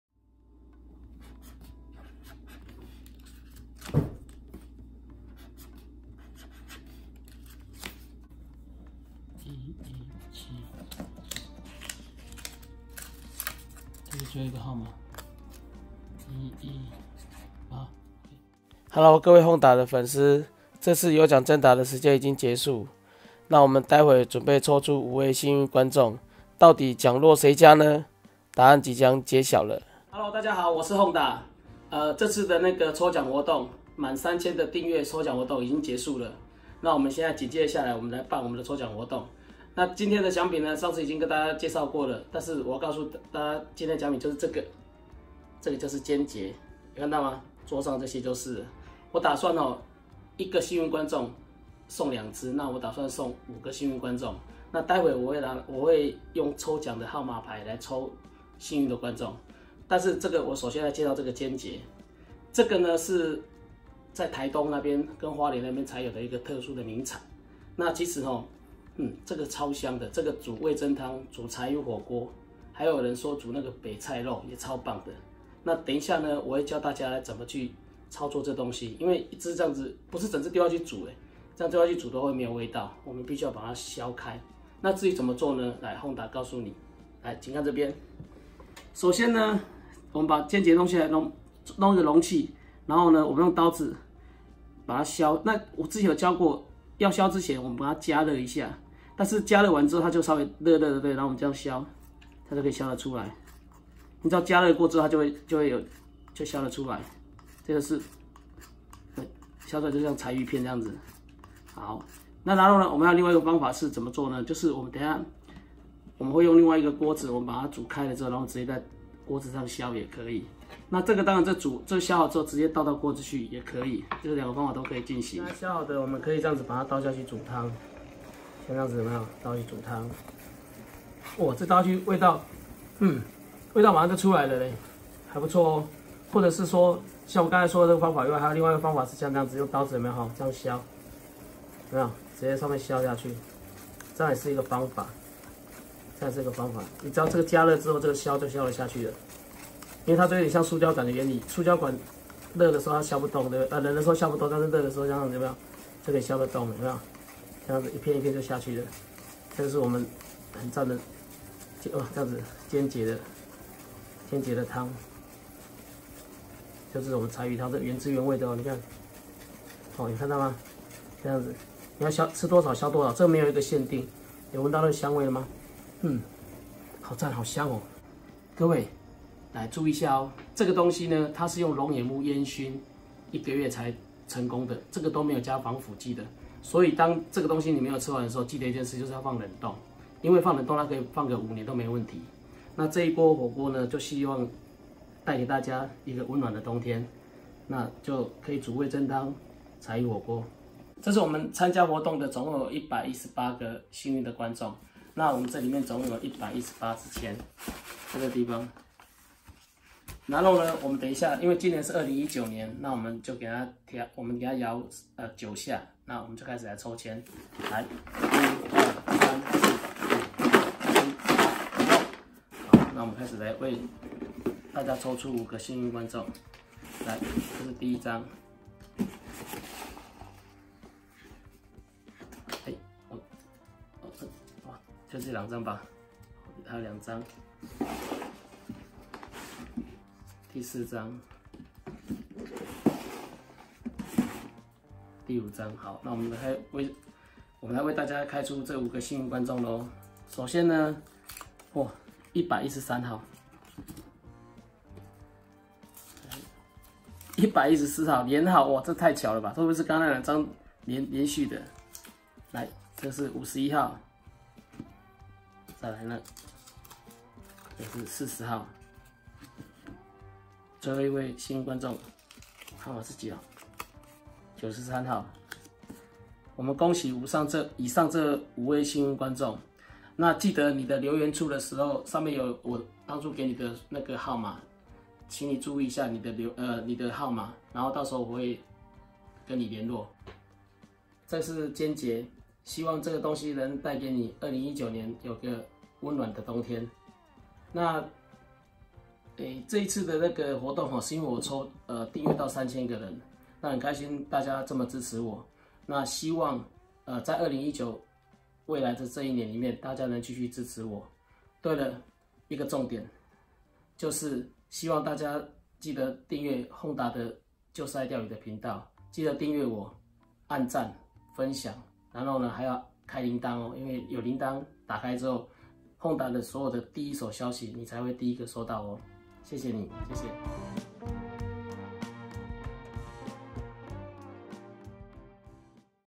如何削鰹節呢？冬天味增湯柴魚火鍋的最佳搭檔 3000訂閱抽獎完成 跟著HONDA去旅行
終於達成3000訂閱目標 謝謝大家的支持 我抽出5位幸運觀眾 每一位幸運觀眾鰹節二隻 請大家注意中獎名單 在留言處留下地址或店對店名單 請各位繼續支持 感謝